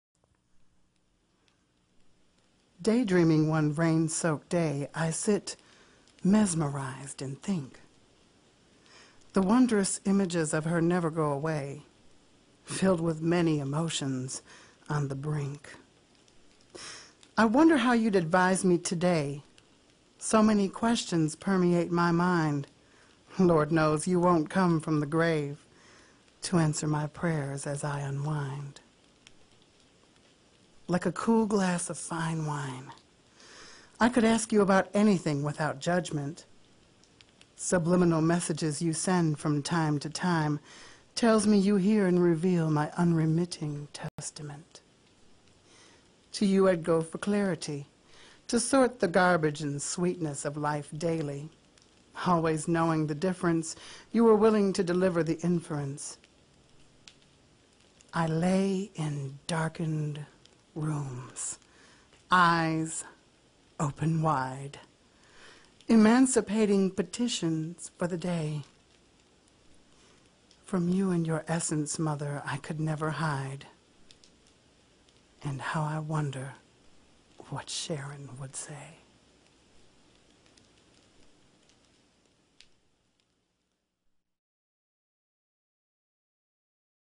I Wonder What Sharon Would Say - Antonio Cassone
From DIVA/CASSONE, a poem I wrote in memory of my mother.  Thought I would release this video marking and celebrating what would have been her 70th birthday.  This marks the first TwitVid - Fo/low @cassone on Twitter [a recording of a poem I wrote in memory of my Mom... today would have been her 70th birthday.  Happy B-Day, Mom!
] posted September 4th 2010 05:17pm from TwitVid to see and hear more works by Cassone - Thanks!